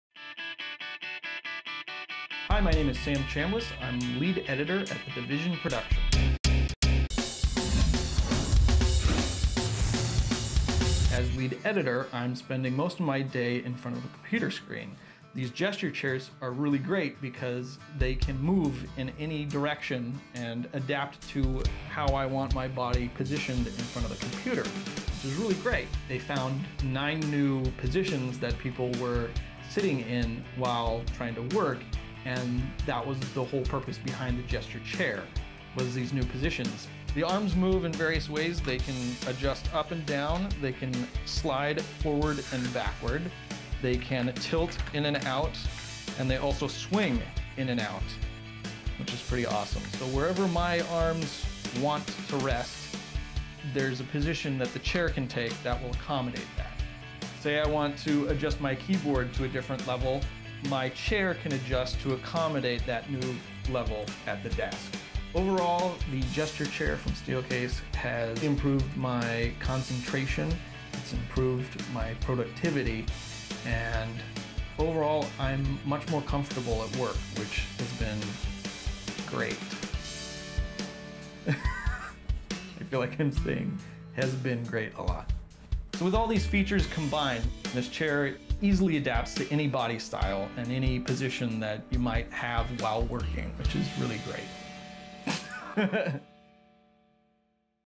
Graveyard Carz Reviews the Steelcase Gesture Chair
Sam at Division TV, makers of Graveyard Carz, lets us know why he loves the Steelcase Gesture Chair.